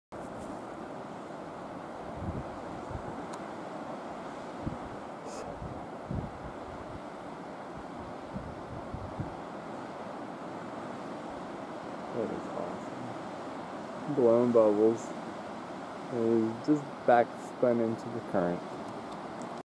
Sea Lion playing in the rapids
California Sea Lion swimming in Seymour Narrows off of Quadra Island, BC.